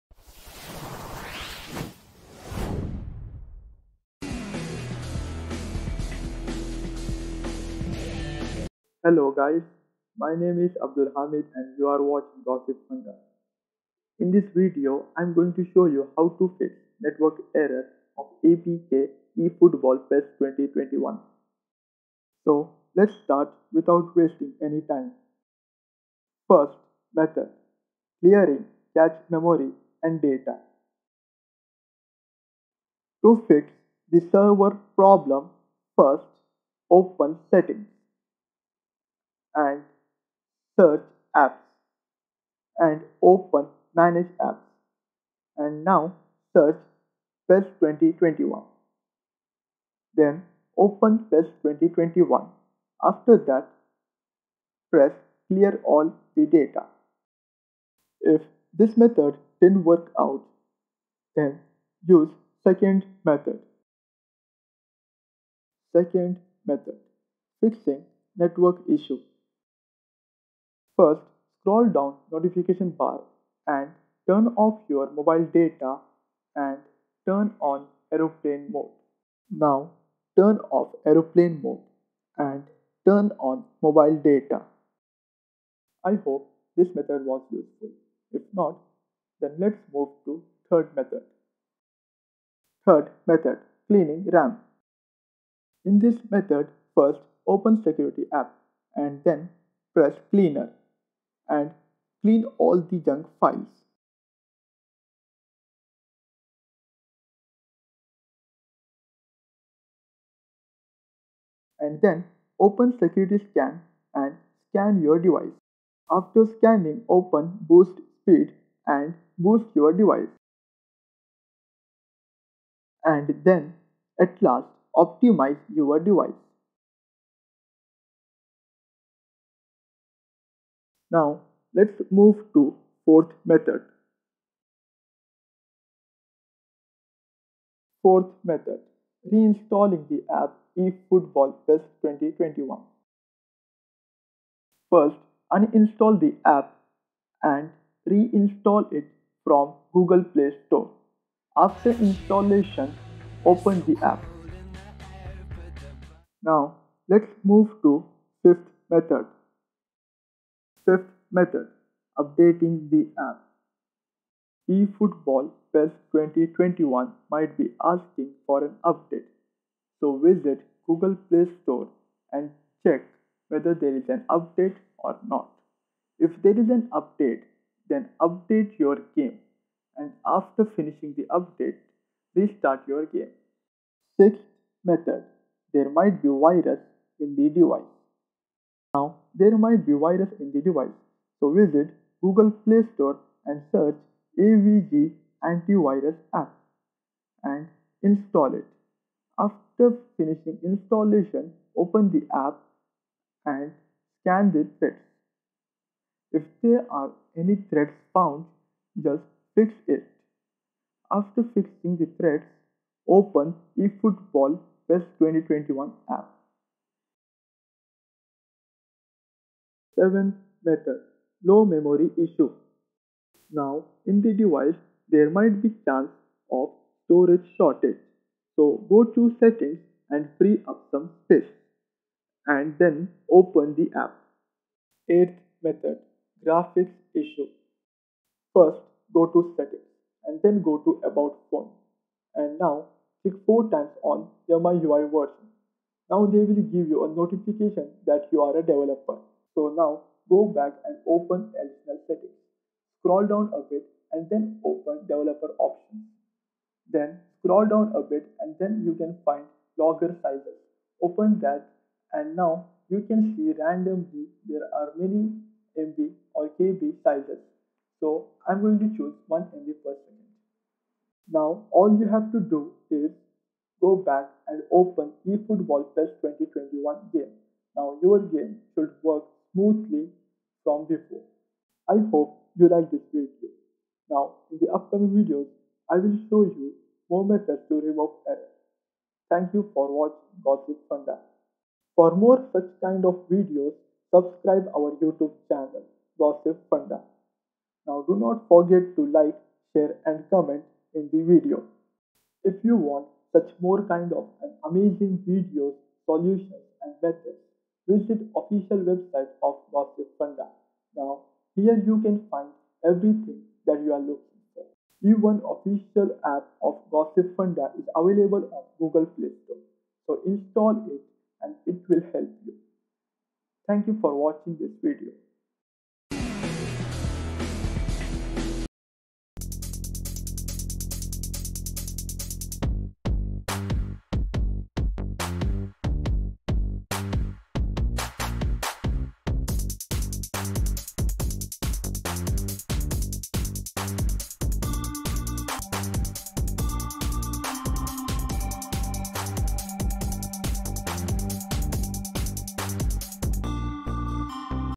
How to Fix PES 2021 C_ETRH_1002 Error Code
8.66:00 - introduction
25.29:00 - 1st Method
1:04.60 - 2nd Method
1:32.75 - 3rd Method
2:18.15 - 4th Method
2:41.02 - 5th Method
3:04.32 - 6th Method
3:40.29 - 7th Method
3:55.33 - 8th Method

1. Link to the short in question
2. Link to your YouTube channel (for ownership verification)